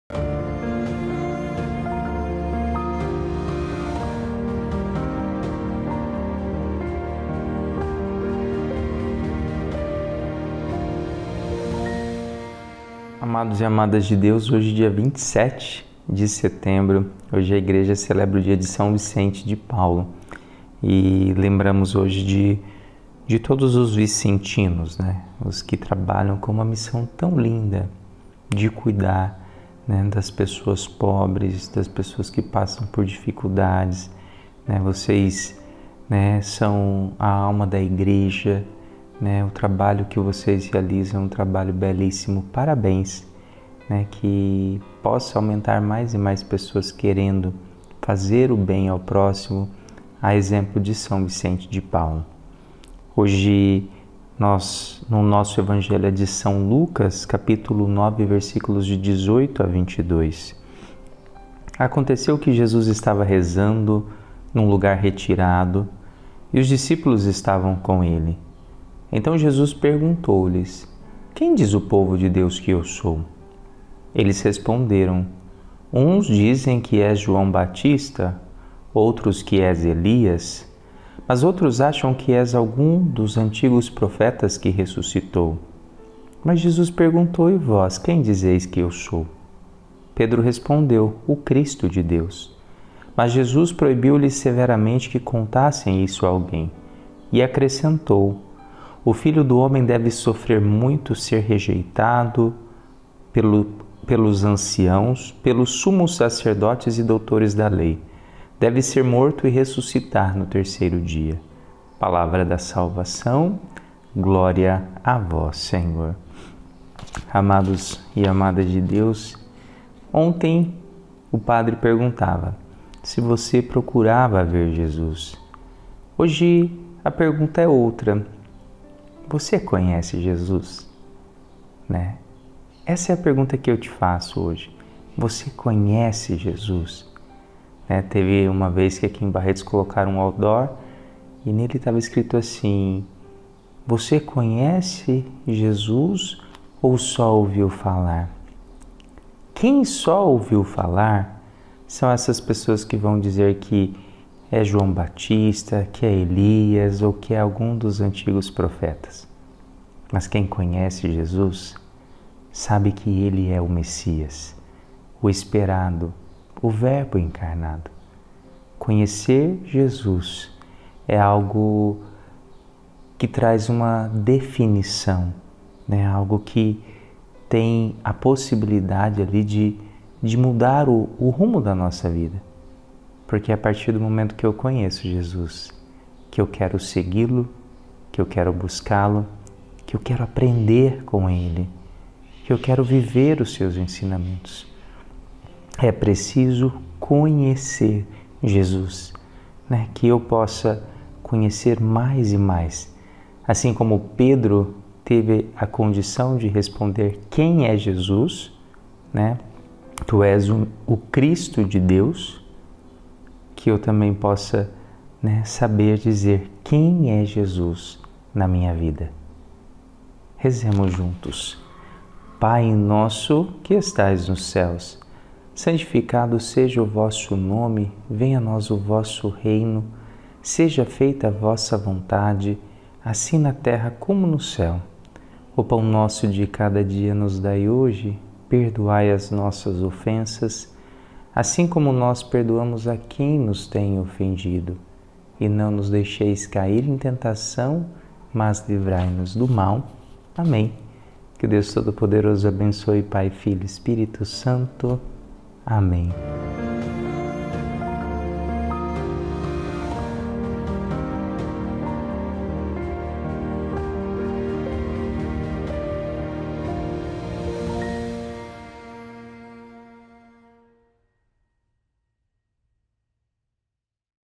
Homilia Diária - Sexta-Feira - São Vicente de Paulo.
Não basta declarar e aceitar que Jesus é o messias ; é preciso rever a ideia a respeito do Messias o qual para construir a nova história enfrenta os que não querem transformações.